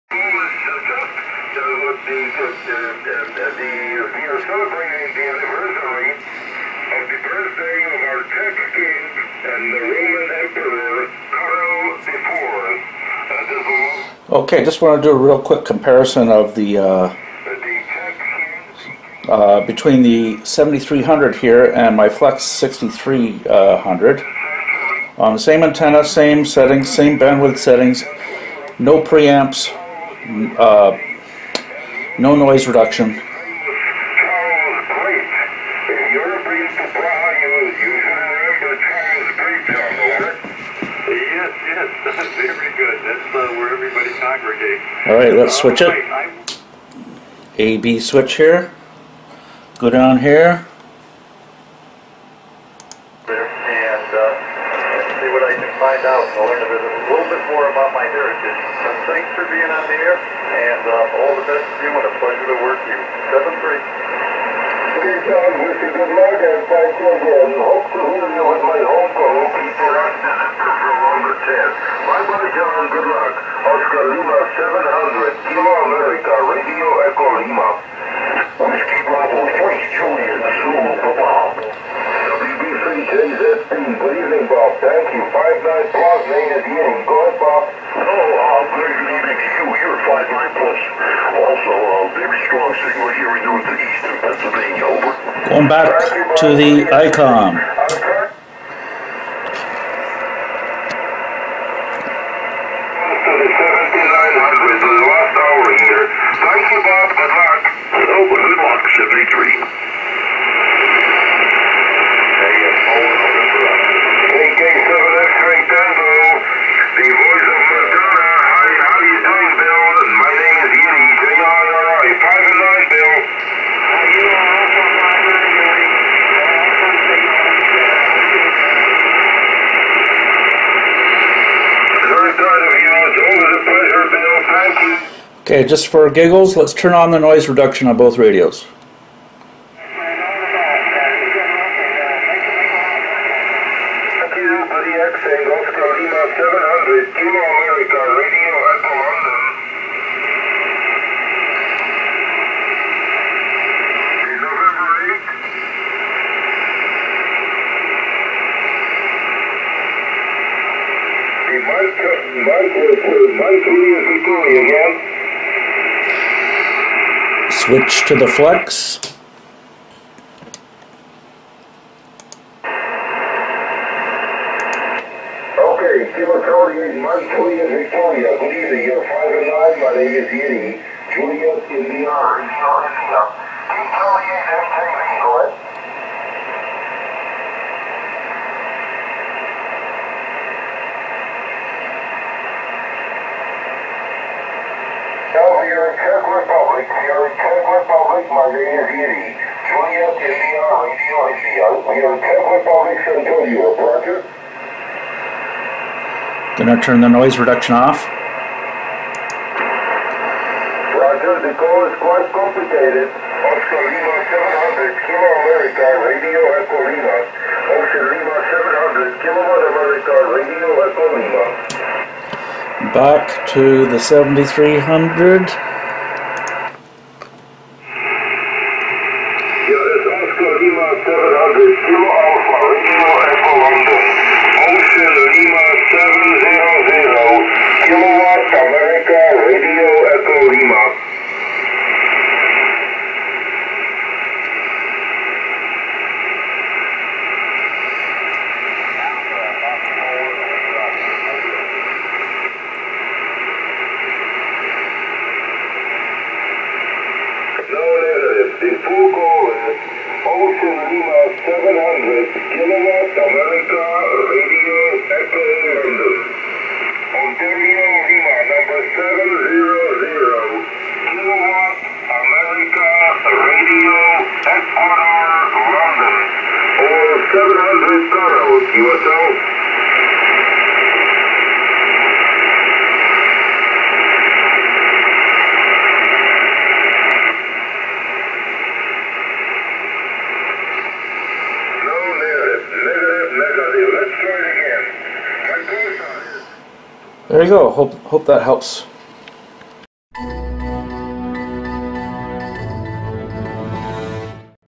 Icom IC 7300 vs Flex 6300 - COMPARISON
Here is a side by side comparison of the Icom 7300 vs Flex 6300. Same antenna, same filter settings, same bandwidth etc. Both using medium AGC settings.

SSB signals on 20 meters using an S9 vertical antenna fed via 9:1 balun.